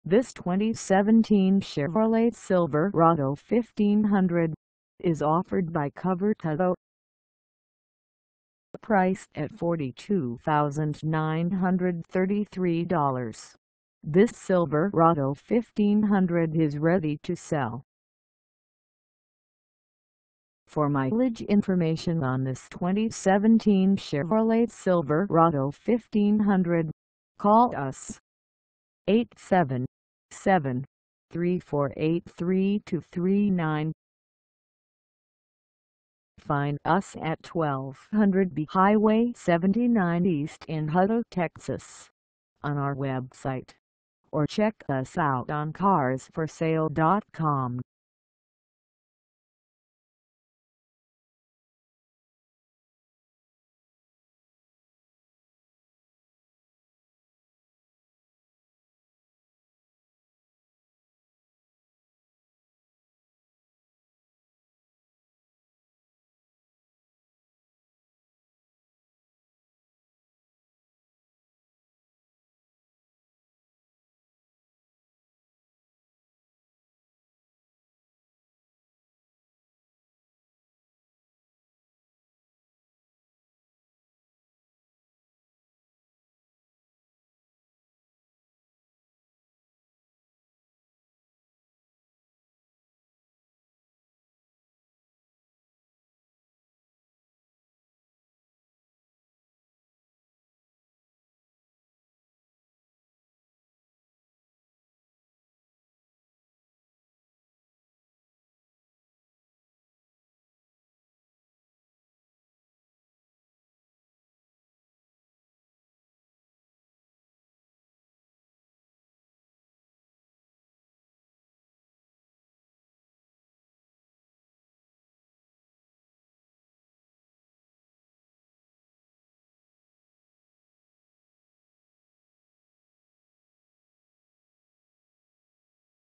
2017 Chevrolet Silverado 1500 LT for sale in Hutto, TX 78634
This 2017 Chevrolet Silverado 1500 LT is for sale in Hutto, TX 78634 at COVERT HUTTO.

Engine: 5.3L V8
Transmission: Automatic 6-Speed
Color: Black
Mileage: 0
Contact COVERT HUTTO 1200 B HWY 79 E Hutto, TX 78634 to test drive this 2017 Chevrolet Silverado 1500 LT today.